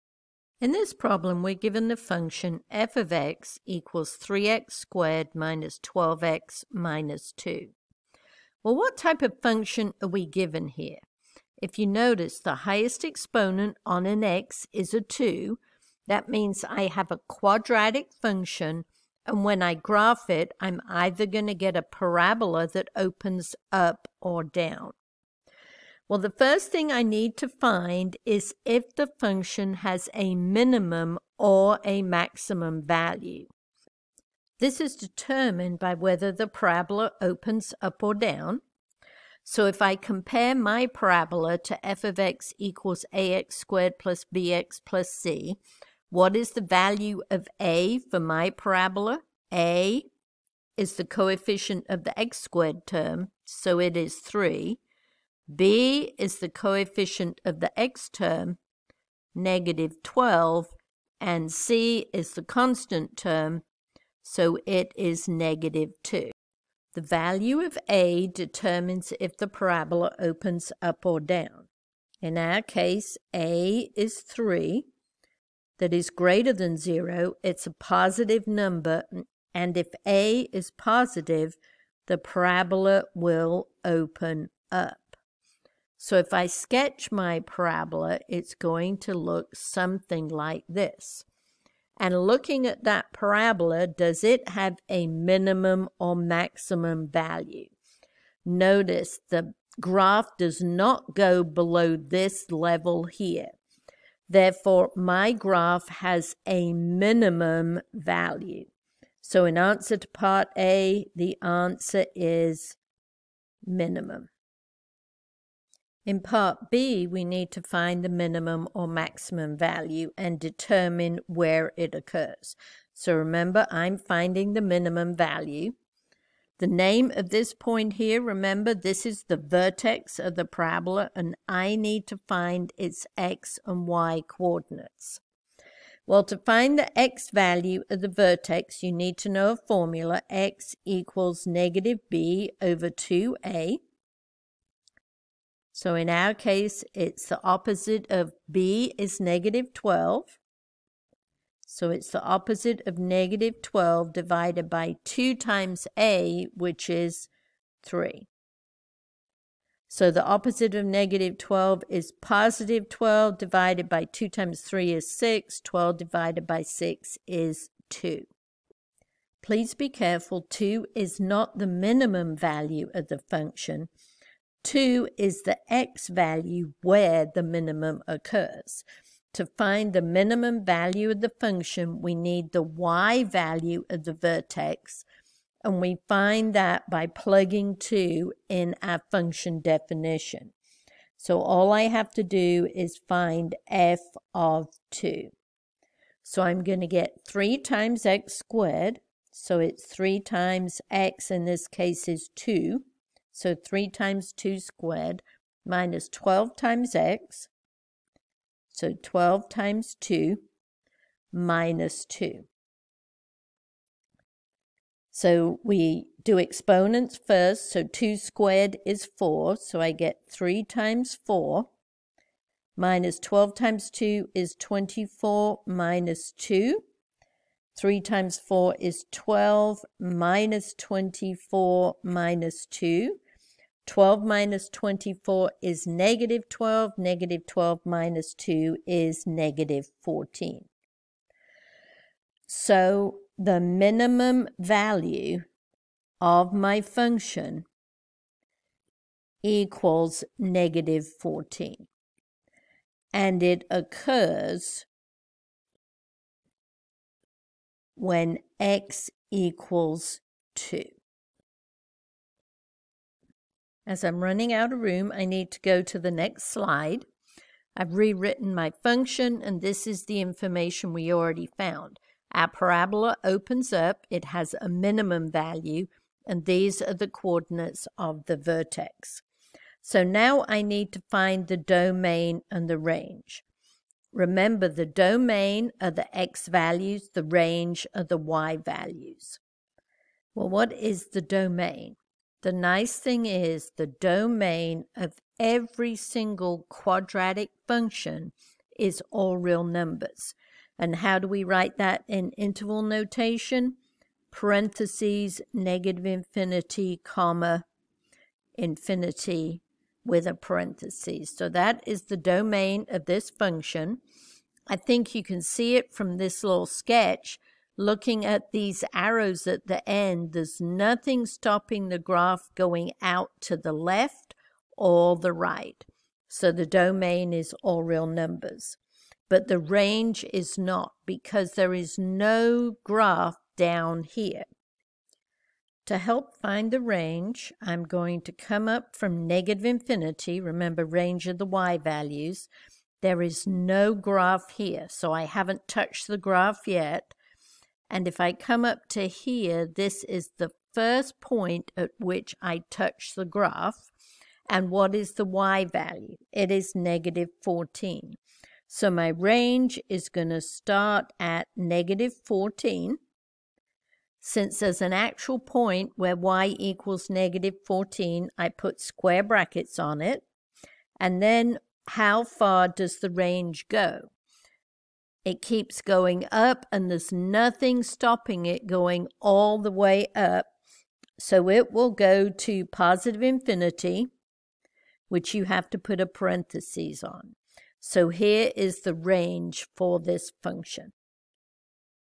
Quadratic Function: Does it Have Max/Min Value, Find this Value, Domain, & Range
The given quadratic function is f(x)=3x^2-12x-2. In order to find if the function has a maximum or minimum value, it is necessary to find if the parabola opens up or down.  As the value of a is positive this parabola opens up so it has a minimum value.  Both coordinates of the vertex are found in order to find the minimum value of the function and where it occurs.  Finally the domain and range of the function are found by looking at a sketch of the function.

Timestamps:
0:00 Introduction
0:38 Does Parabola Open Up or Down?
1:33 Max or Min?
2:10 Find Vertex
4:41 Domain and Range